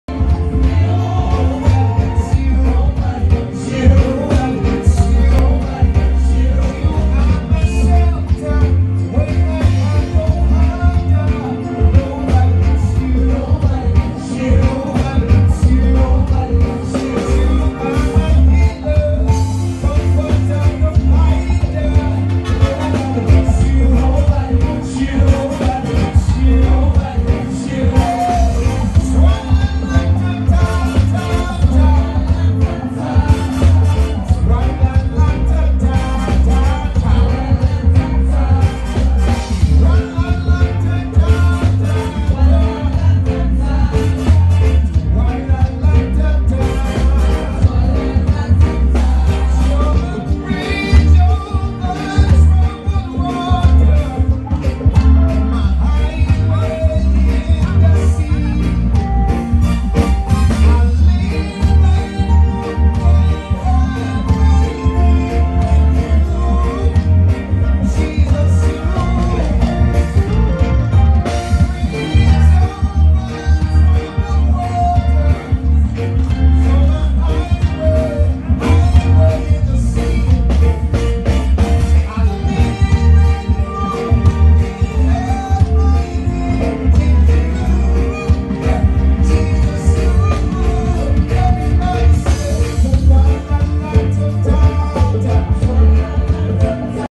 Hot Gospel Reggae Jam with Akesse Brempong @ Mukyia Delight, Patasi | Samuel Gyasi-Darko on drums
With Akesse Brempong (The Lion), Barima Okyere, Pilato, Big Bro Achiekeyz, EddieKeyz.